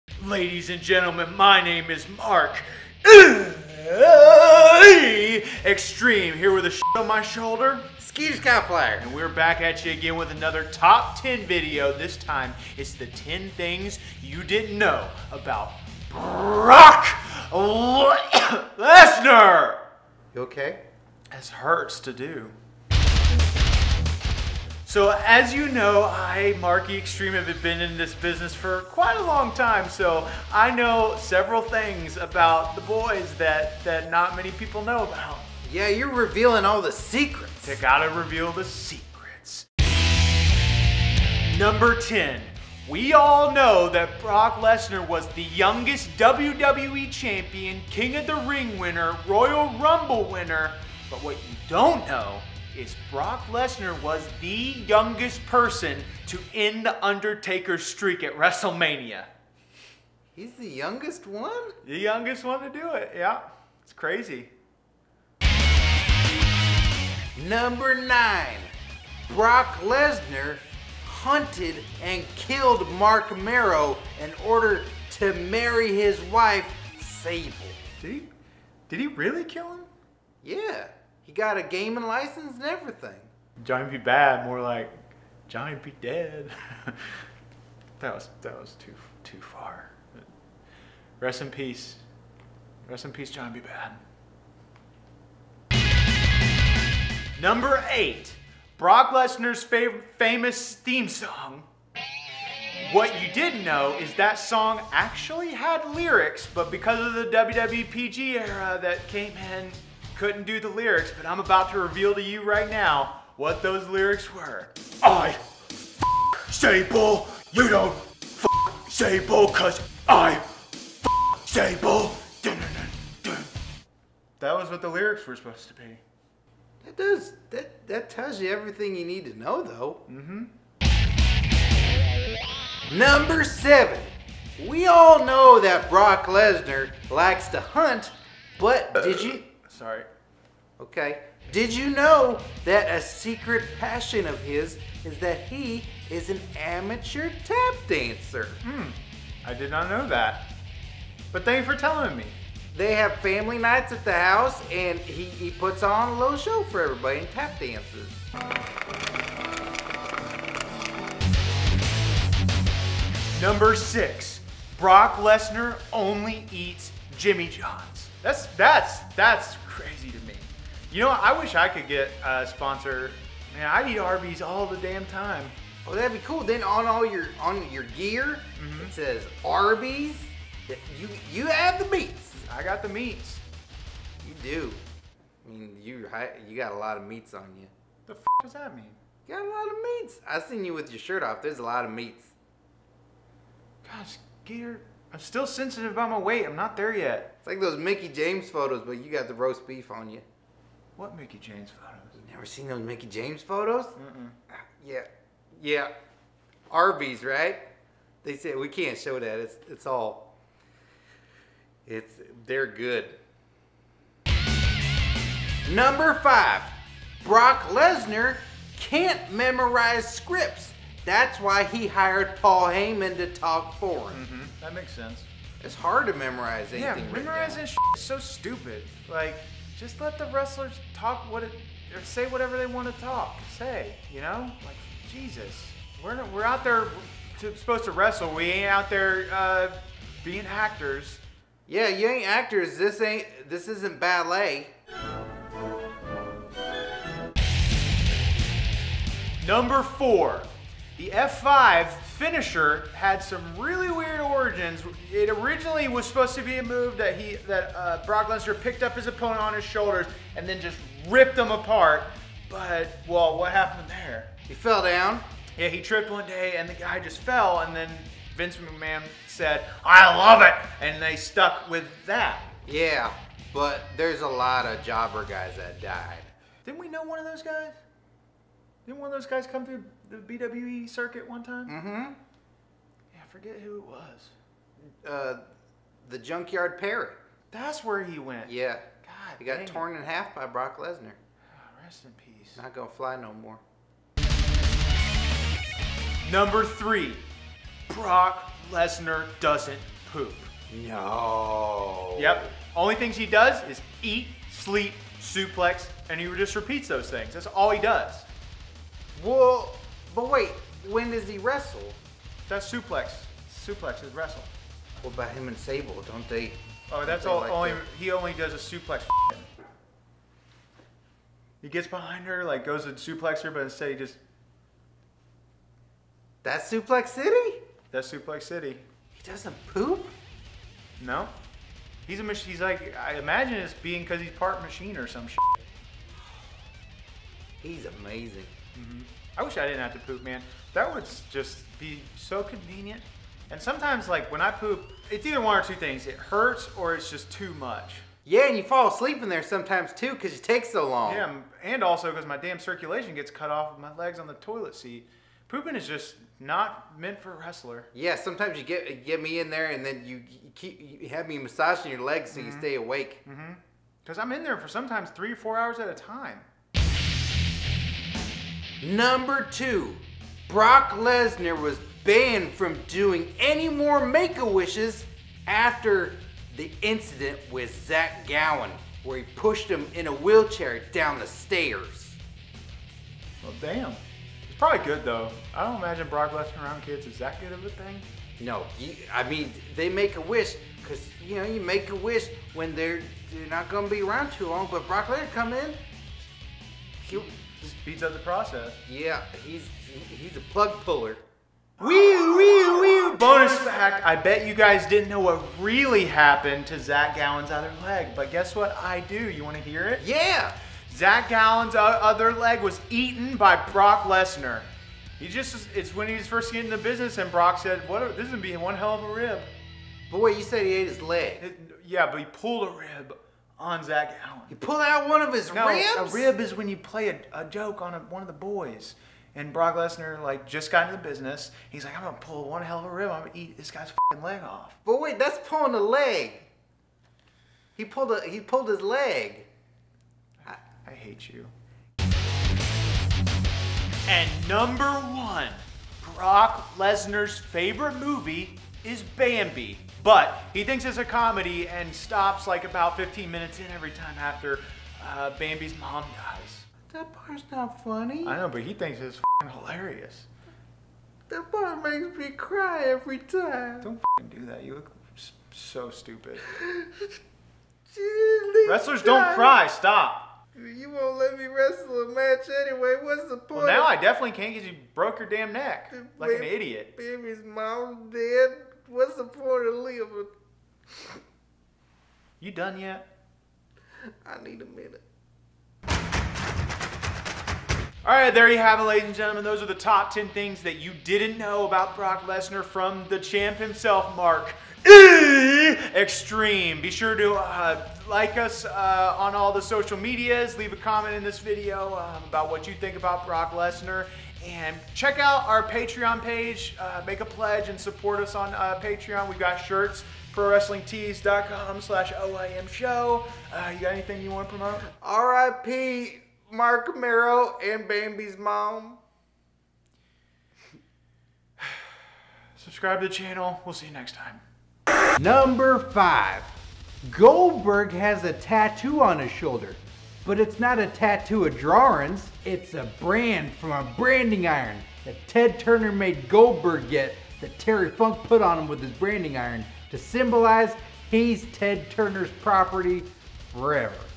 Top 10 Things You Didn't Know About Brock Lesnar
Mark E Xtreme and Skeeter Skyflyer reveal the Top 10 Things You Didn't Know About Brock Lesnar.

Produced by: Shane Hartline and Scott Narver.